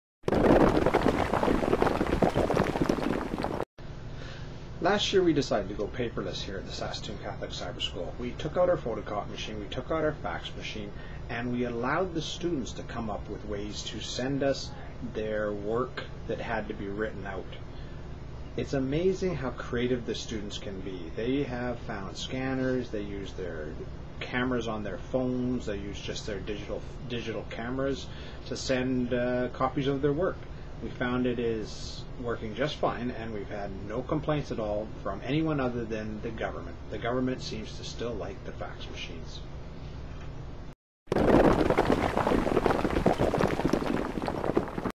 TADO Vids Paperless Cyber School (Easier than you think)
It is a lot easier than you think to go paperless in schools... ask the students for ideas  (Old video 2008)  We have been paperless for 3 years now...so easy.